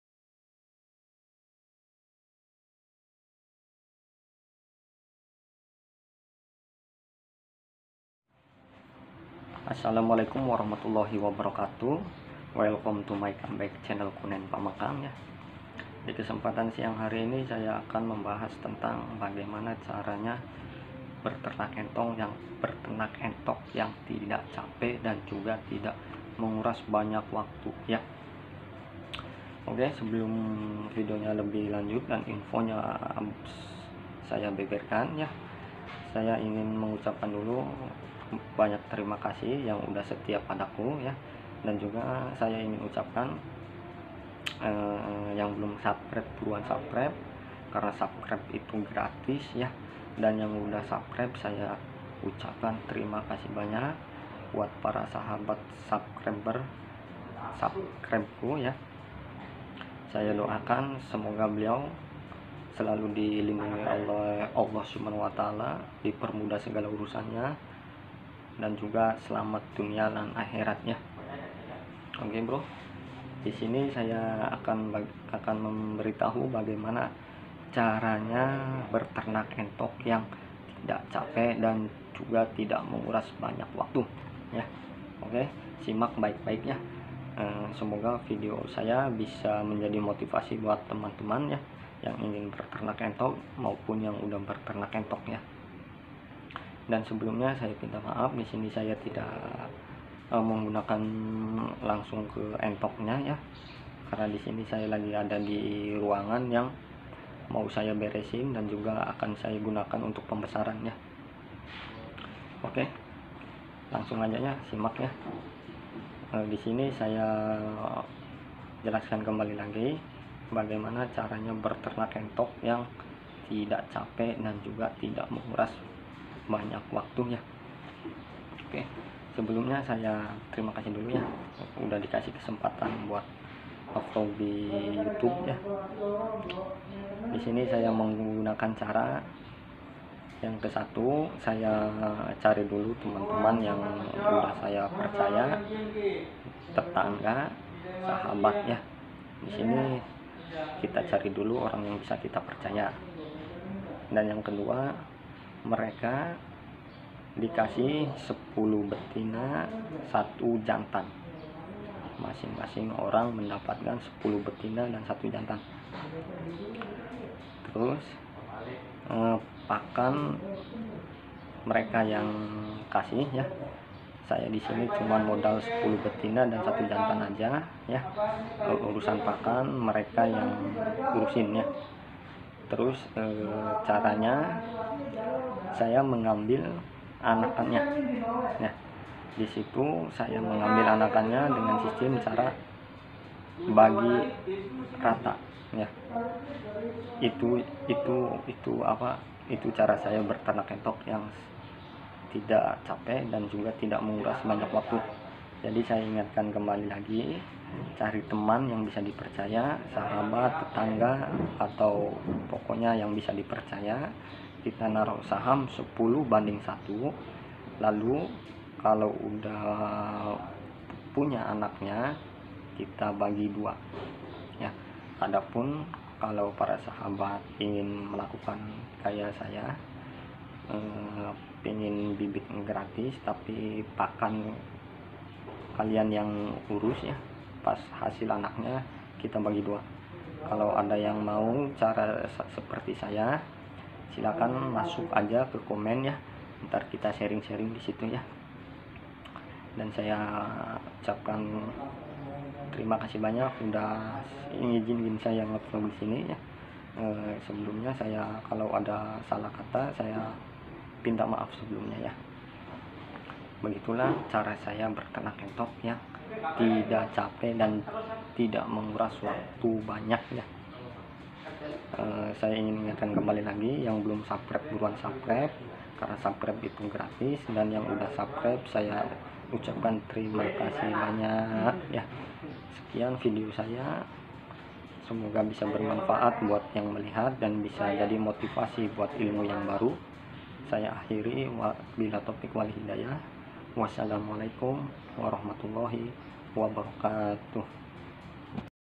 CARA BETERNAK ENTOK RAMBON YANG SUKSES DAN MENGHASILKAN
cara beternak entok yg sukses#cari sahabat temen tetangga yg bisa di percaya#kasih bibit 10 betina 1 jantan#pakan mereka yg tanggung dan hasil dari Meri nya kita bagi rata#entok rambon silver,klawu,salju,dragon#kunen pamengkang#